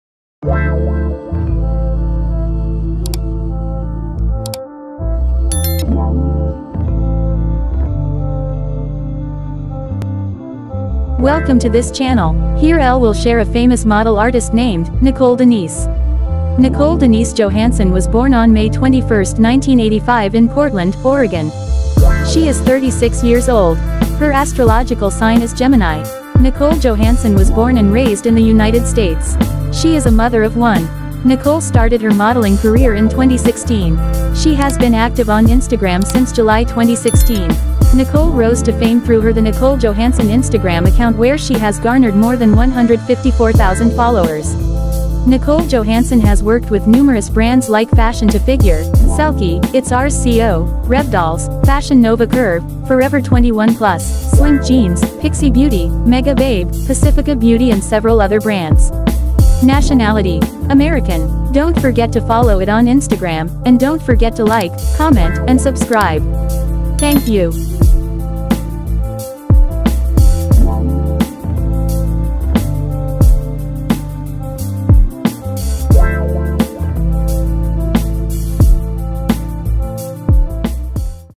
Nicole Denise Johansson 🇺🇸...| American Gorgeous Plus Size Model | Curvy Fashion Model | Biography
Original content owner: @Nicole Denise Johansson
Follow on Instagram - @thenicolejohansson

Nicole Denise Johansson is an American Plus-sized Model, Fashion Model, Make-up Artist, Hair Stylist, Body Positive Advocate, Influencer and Instagram Star who wants women all around the world to know that they were created beautifully and wonderfully. Nicole Denise Johansson was born in Portland, Oregon. Her astrological sign is Gemini. Nicole Johansson was born and raised in the United States.